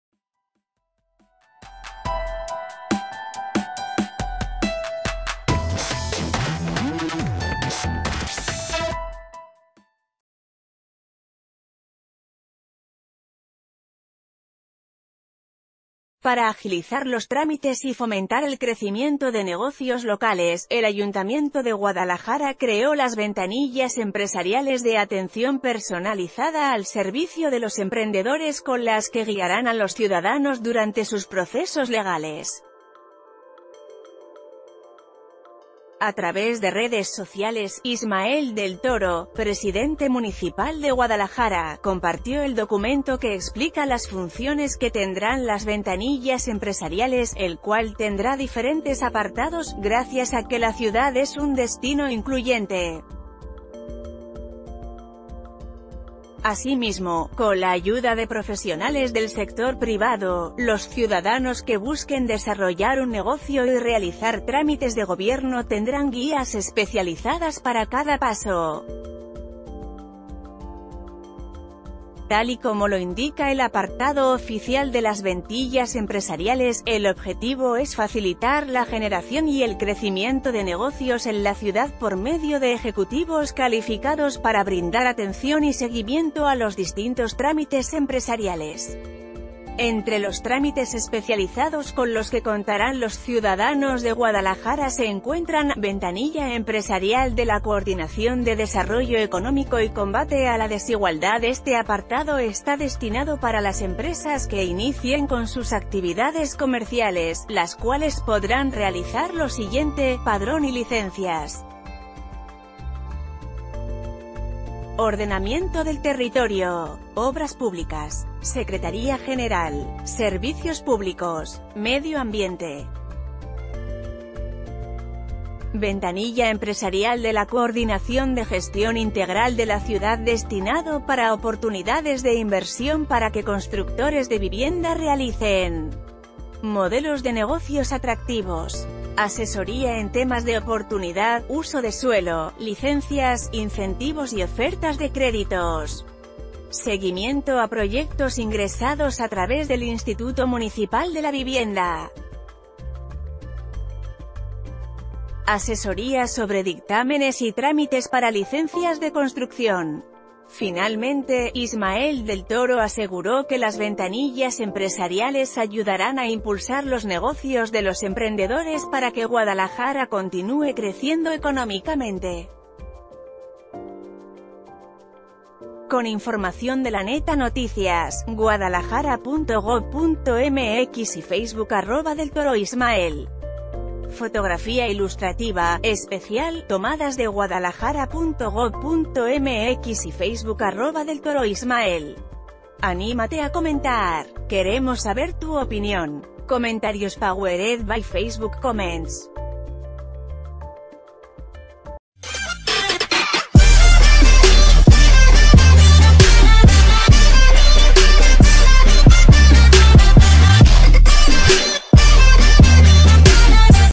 Del Toro presenta Ventanillas Empresariales, el sistema para apoyar negocios
Para agilizar los trámites y fomentar el crecimiento de negocios locales, el Ayuntamiento de Guadalajara creó las Ventanillas Empresariales de Atención Personalizada al Servicio de los Emprendedores con las que guiarán a los ciudadanos durante sus procesos legales.